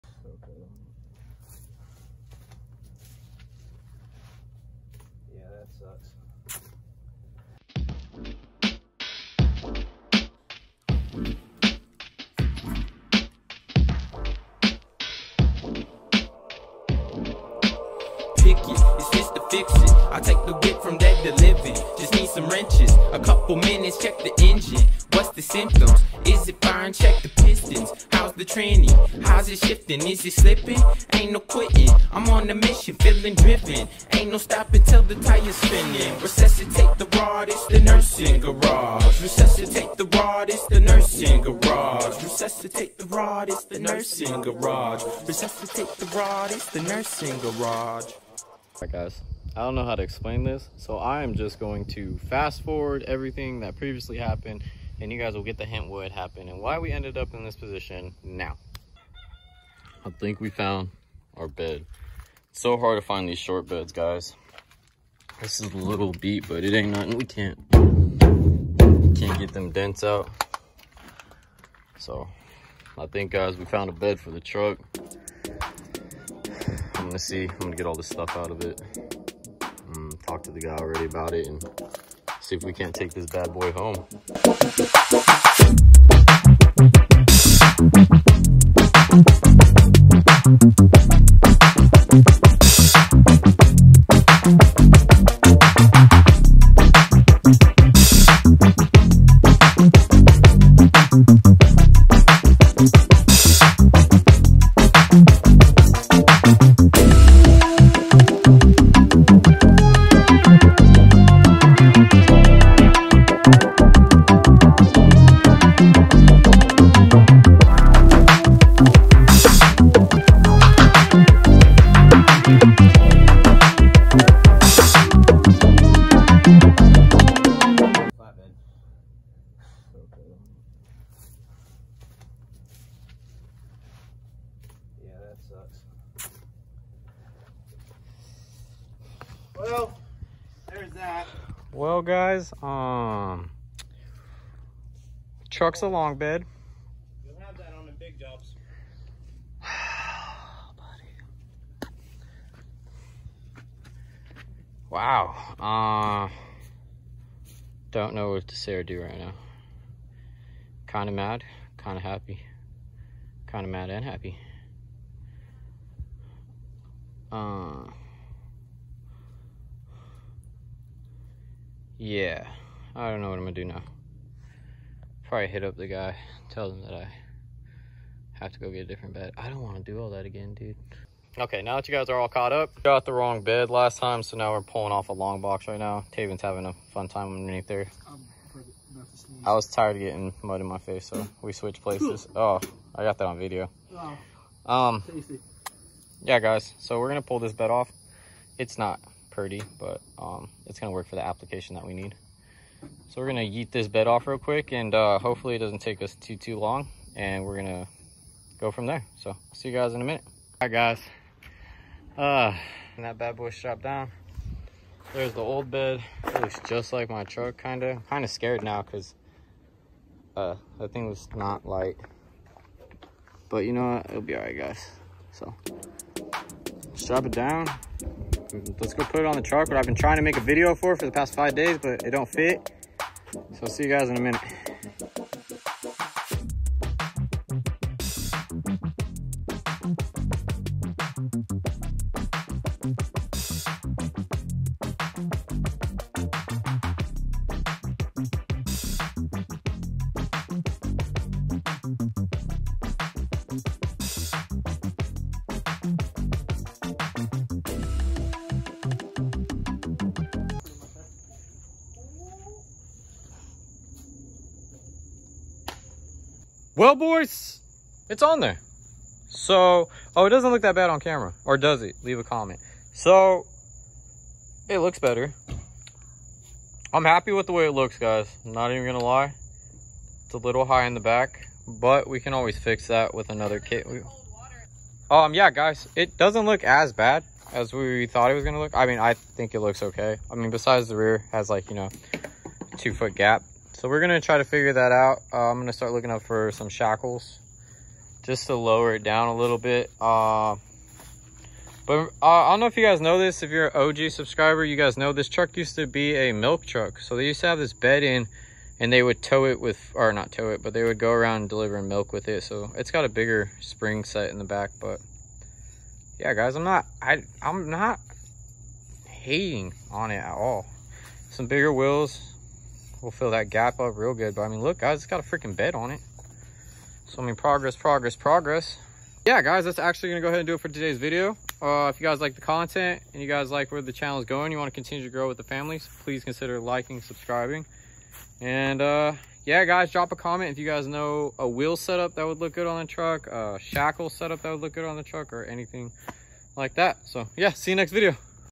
Making the biggest rookie mistake ever! Always measure!!!
What’s up guys thanks for coming back to the channel! We have a situation on our hands that we have to get fixed! Hope you guys enjoy the video!